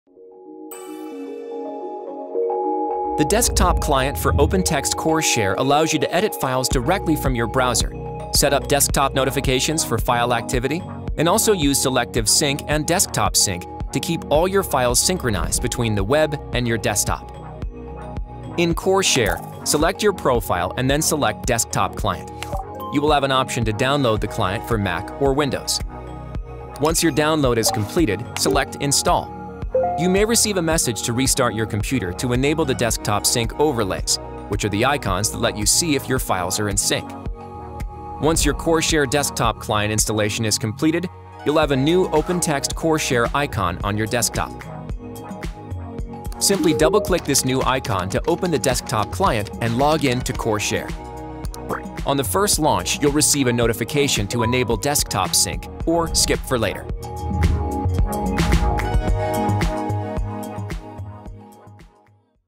Download the Desktop Client | OpenText Core Share
See how the OpenText Core Share desktop client allows you to control notifications for account activity, instantly edit files from your browser using applications on your desktop, and set up selective sync so you can keep everything in check between the cloud, your desktop and mobile devices.

OpenText Core Share helps enterprises securely and easily share, collaborate and store content in the cloud.

or the Core Share Community Forum.